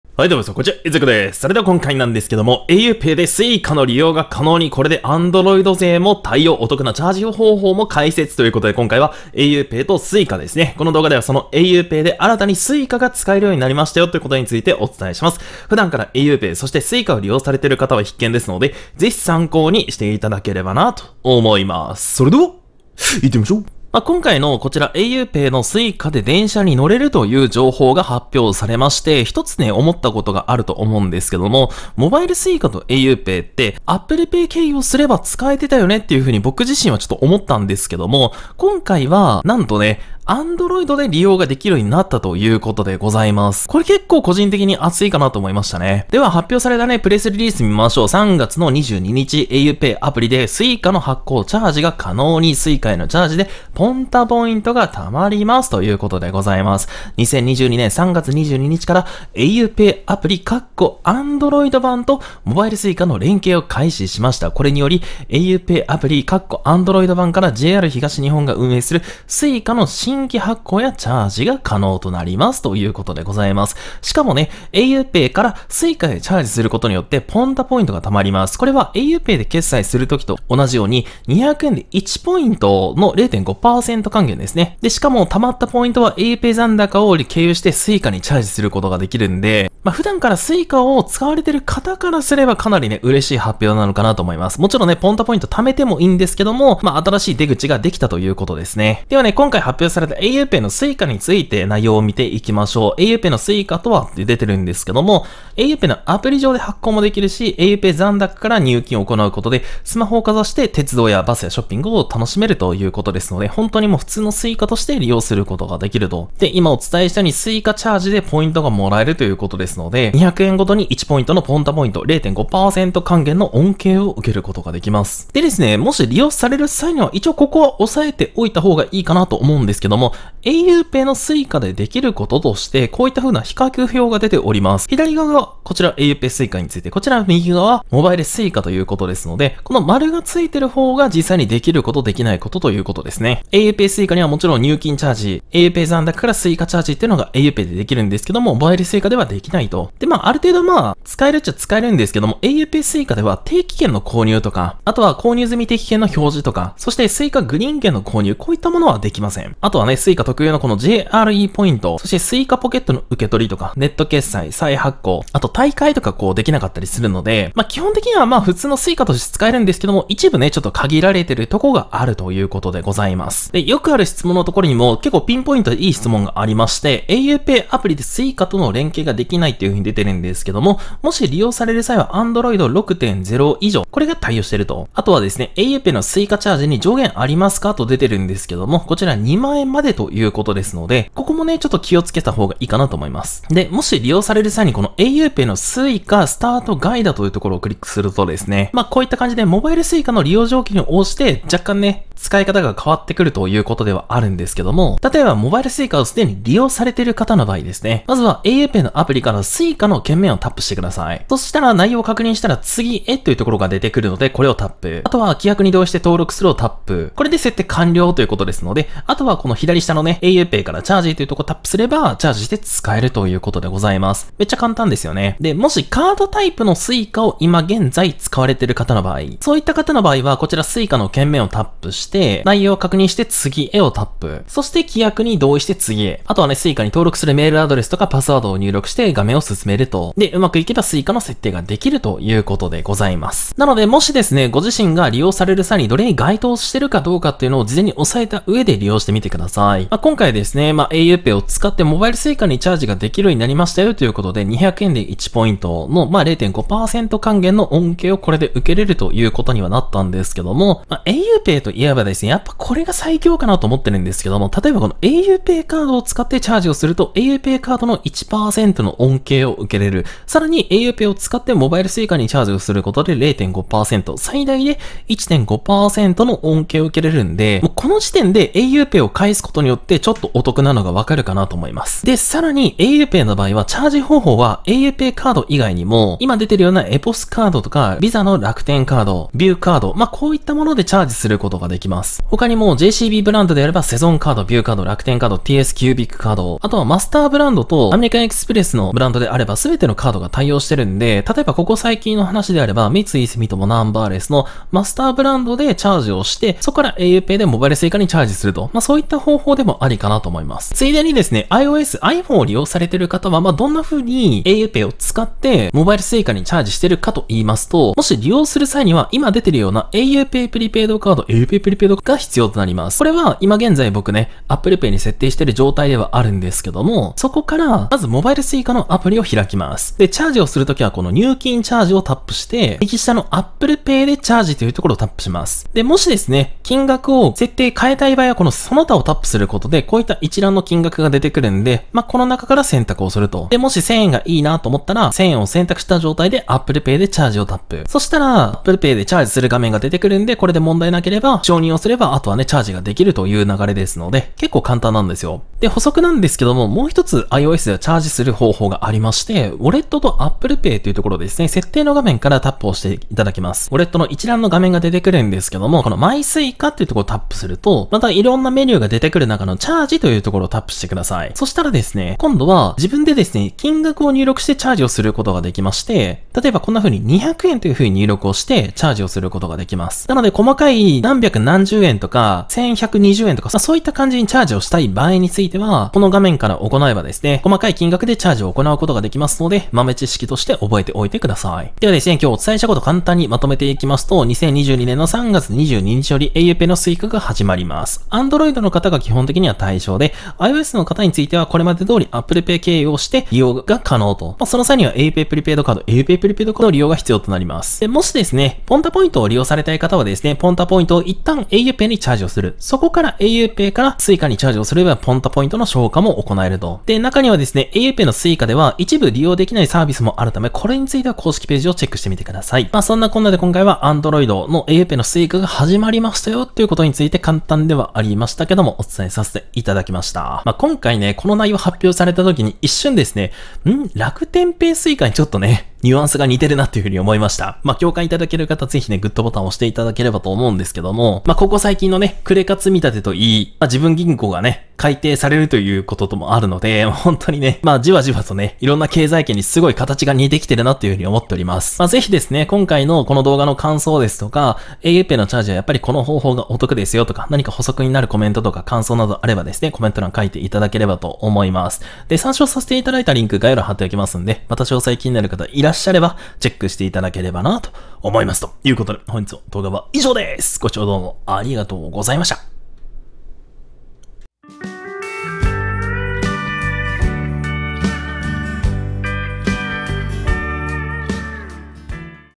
auPAYでSuicaの利用が可能に！これでAndroid勢も対応！お得なチャージ方法も解説
【PR】 auPAY×Suica

使用機材やデスク回りなどはこちらで紹介しています